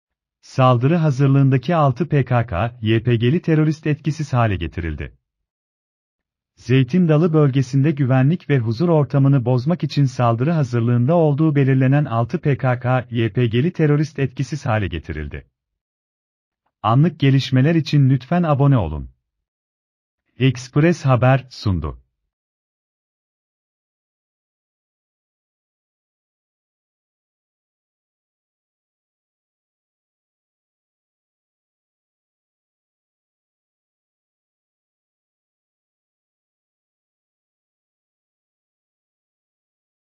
SON DAKİKA: Saldırı hazırlığındaki 6 PKK/YPG’li terörist etkisiz hale getirildi
Zeytin Dalı bölgesinde güvenlik ve huzur ortamını bozmak için saldırı hazırlığında olduğu belirlenen 6 PKK/YPG’li terörist etkisiz hale getirildi.